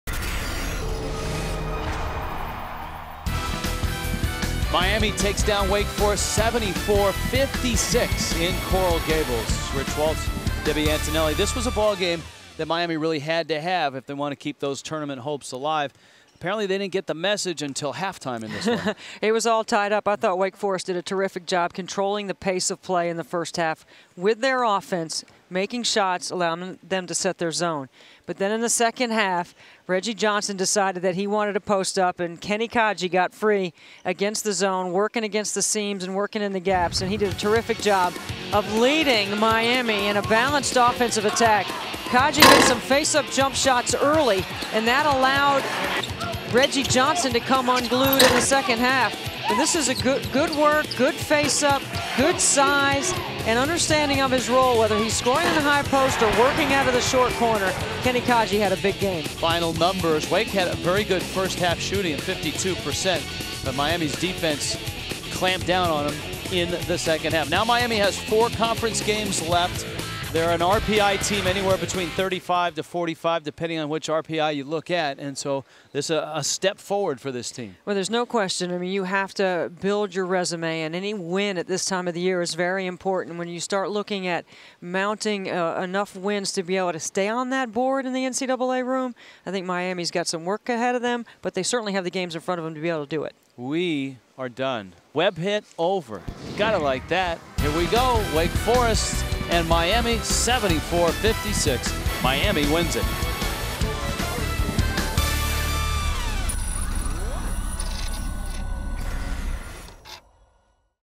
Wake Forest vs Miami Men's Basketball Highlights
Durand Scott and Kenny Kadji each scored 18 points as Miami-Florida downed Wake Forest 74-56 in an Atlantic Coast Conference matchup at the BankUnited Center.